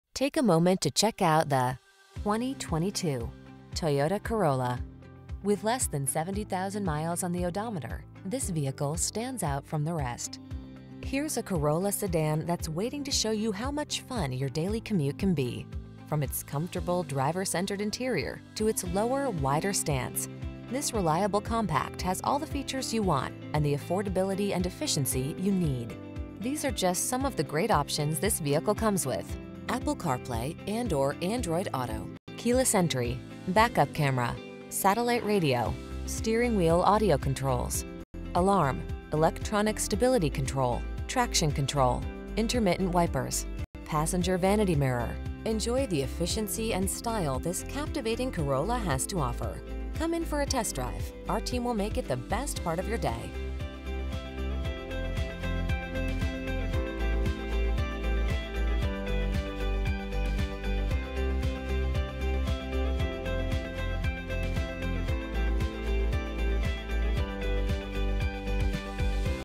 2022 Toyota Corolla LE Birmingham, Hoover, Alabaster, Vestavia Hills, Helena AL
Pre-Owned 2022 Toyota Corolla LE available at Tameron Hyundai located in 1595 Montgomery Hwy, Birmingham, AL, 35216. Servicing the Birmingham, Hoover, Alabaster, Vestavia Hills, Helena area.

Take a moment to check out the  2022  Toyota Corolla.  Here's a Corolla sedan that's waiting to show you how much fun your daily commute can be. From its comfortable, driver-centered interior to its lower, wider stance, this reliable compact has all the features you want and the affordability and efficiency you need.  These are just some of the great options this vehicle comes with: Apple Carplay®/Android Auto®, Keyless Entry, Back-Up Camera, Satellite Radio, Steering Wheel Audio Controls, Alarm, Electronic Stability Control, Traction Control, Intermittent Wipers, Passenger Vanity Mirror. Enjoy the efficiency and style this captivating Corolla has to offer. Come in for a test drive. Our team will make it the best part of your day.